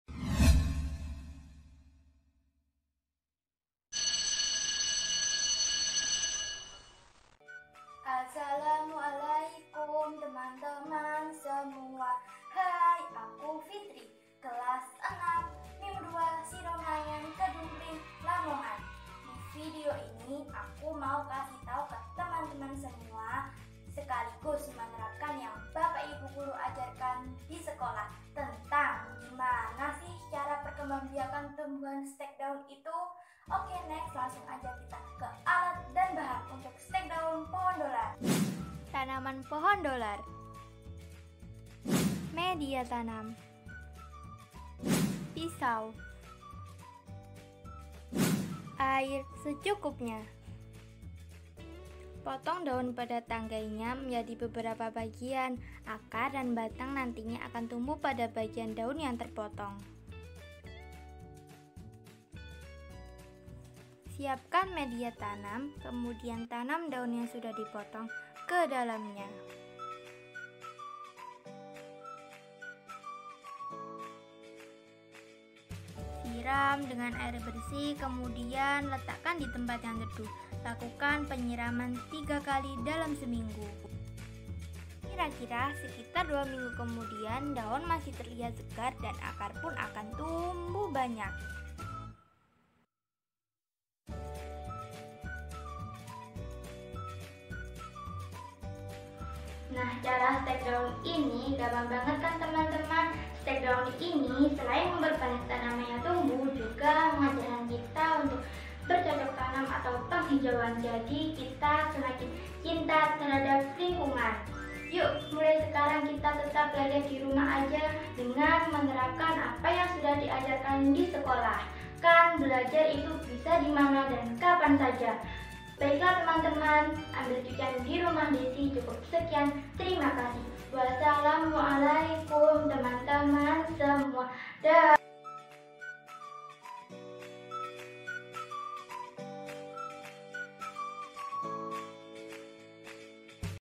VLOG PEMBELAJARAN IPA VEGETATIF BUATAN DENGAN STEK DAUN
Kegiatan vlog pembelajaran yang dilakukan siswi  MI MUHAMMADIYAH 2 SIDOMLANGEAN, Alifia Azahra S di tengah kondisi pandemi Covid-19. Inilah cara kreatif siswi cantik ini. Simak video lengkapnya